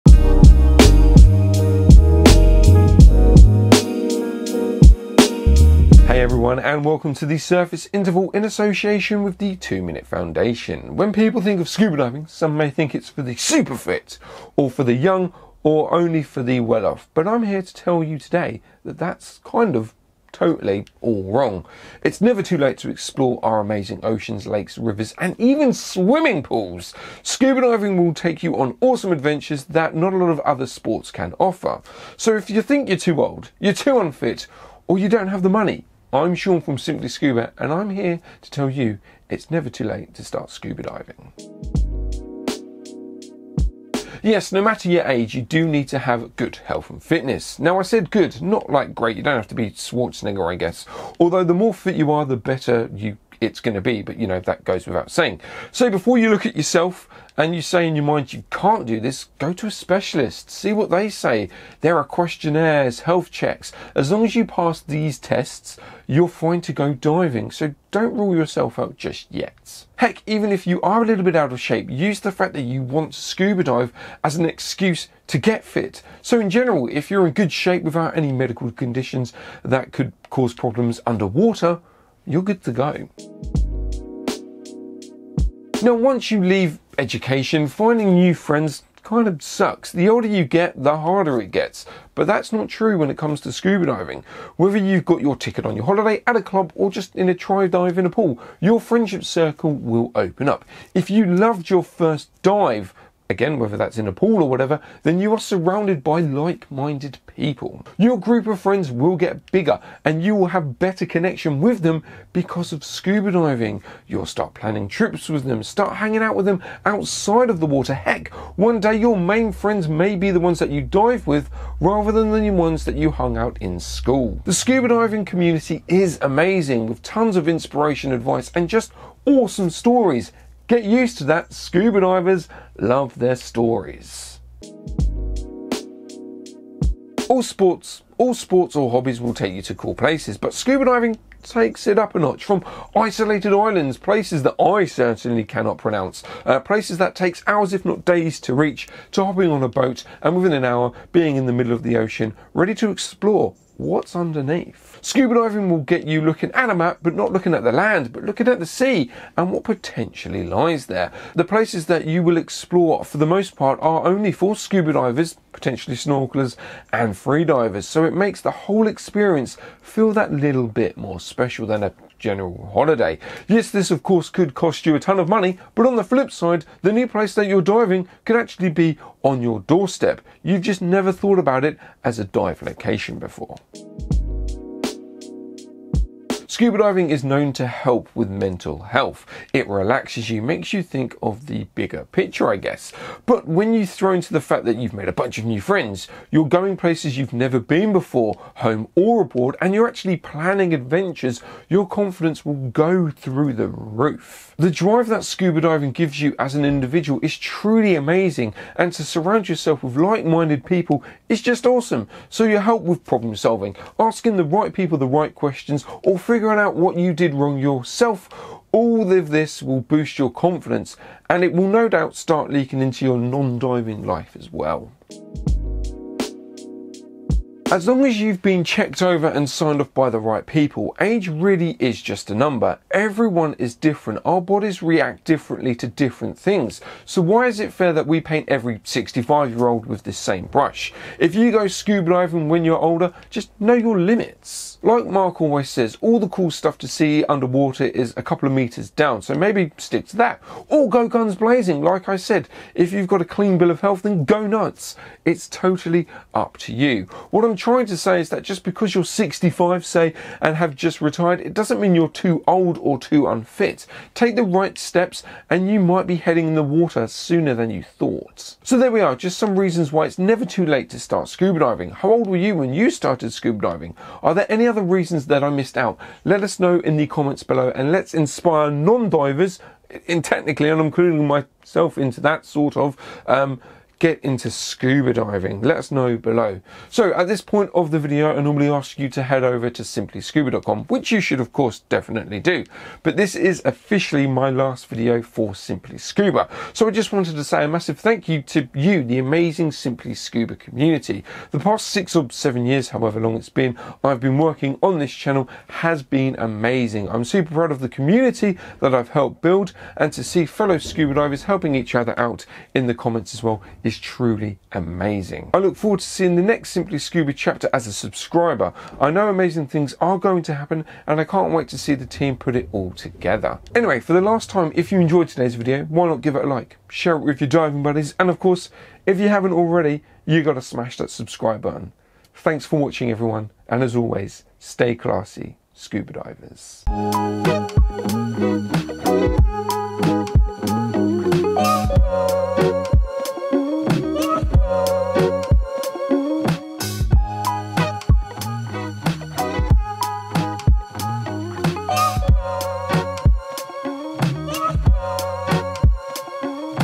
It’s Never Too Late To Start Scuba Diving
It’s never too late to explore our amazing oceans, lakes, rivers and even swimming pools! Scuba diving will take you on awesome adventures that not a lot of other sports can offer.

Chapters
00:00 - Hey
00:42 - Health & Fitness
01:24 - Meet People
02:15 - Go To New Places
03:05 - Boost Your Confidence
03:46 - Age Is Just A Number
04:28 - Farewell From Shaun